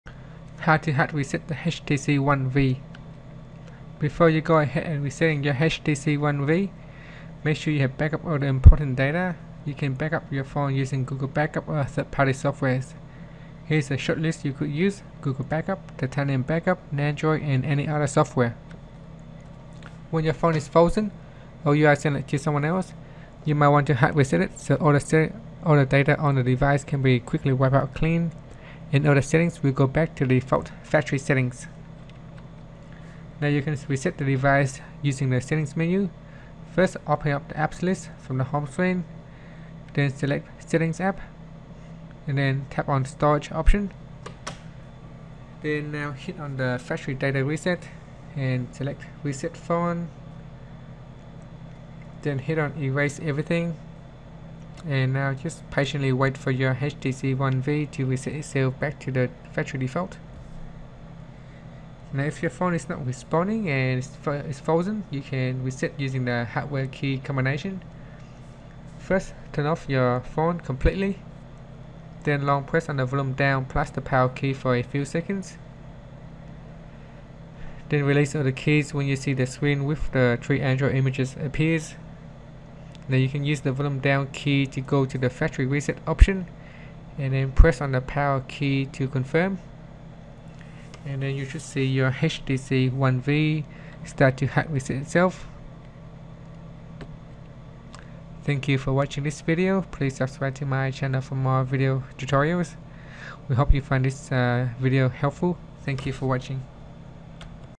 How to Hard Reset HTC One V
Quickly hard reset teh HTC One V back to the factory default settings.